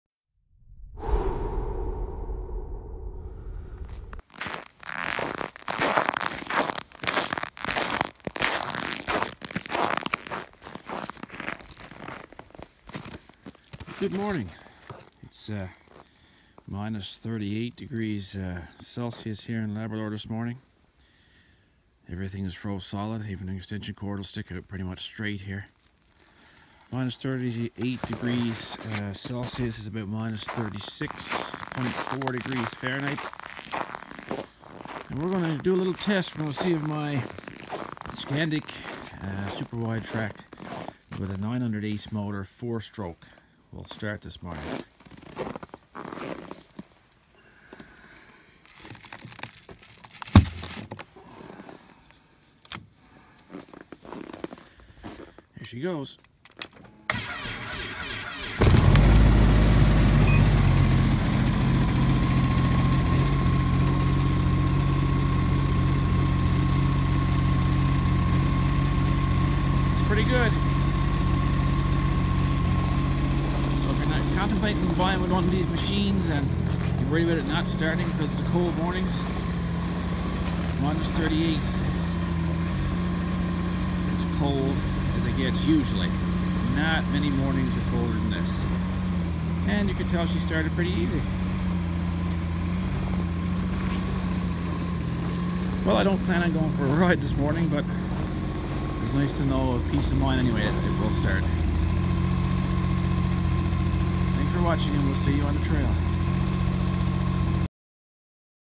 -38 DEGREE STARTUP of SKANDIC SWT (900 ACE - FOUR STROKE)
On the VERY chilly morning I attempt to start up my SKANDIC SWT 900 ACE, Four Stroke engine. It was the coldest morning on record that I attempted to do so. and it started with no problem at all!!!